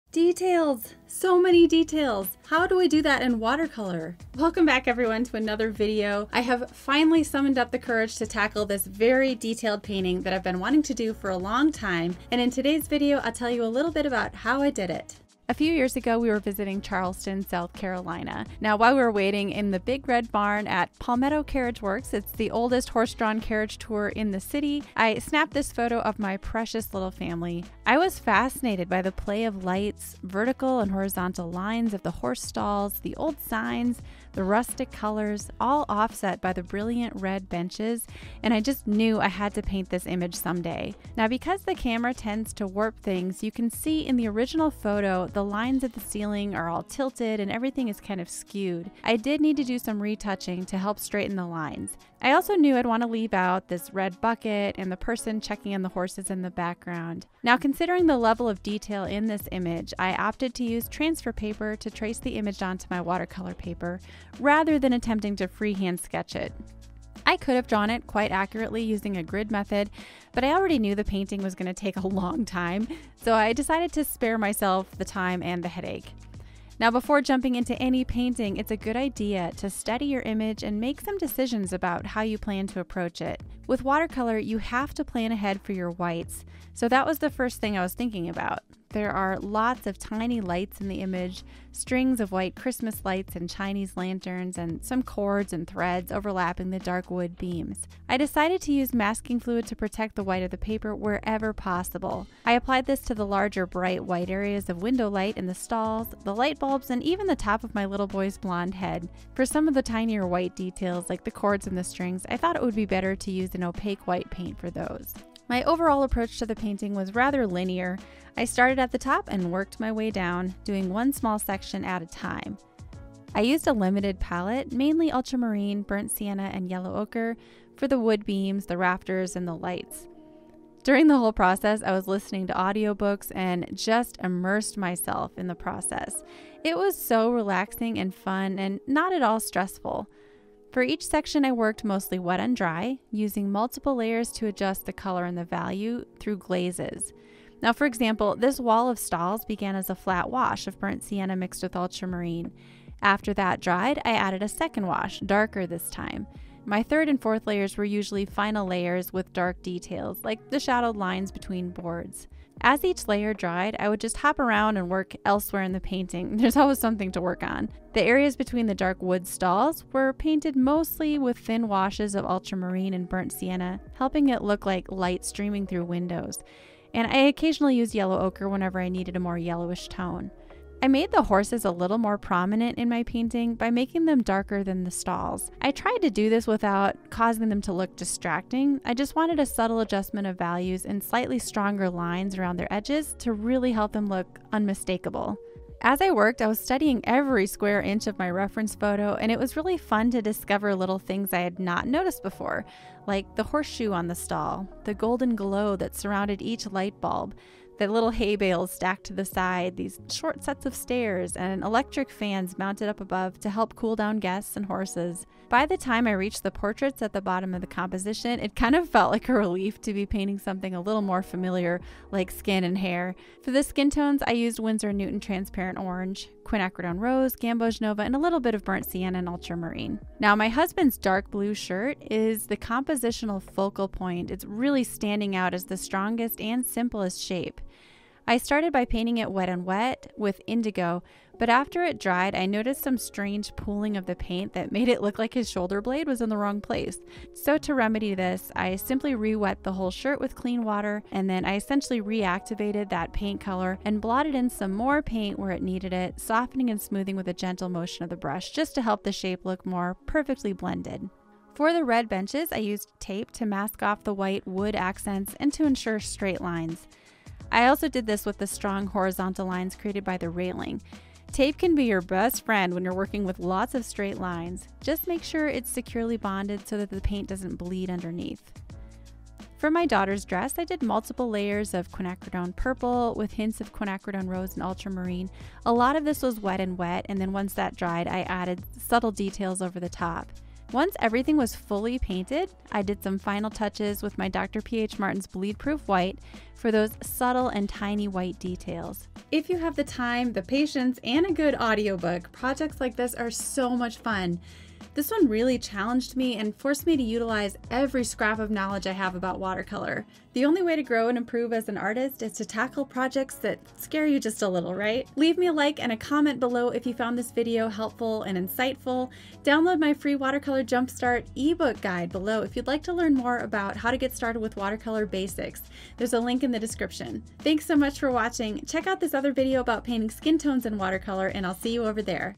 How I paint from a DETAILED photo in watercolor
In this video I'm sharing my process for painting this complex and detailed family portrait!
Learn some tips and techniques YOU can use in your next watercolor painting!
Learn to paint landscapes and so much more by becoming a Watercolor Mastery Member!
If you enjoy this video, please give it a 👍!

Reference Photo (my own)
Here is a list of supplies used in this video:
- Daniel Smith Extra Fine Watercolors
- Holbein Watercolors
- Washi Tape
- Masking Fluid

- Camera Canon 80D with 18-135mm Lens ➡️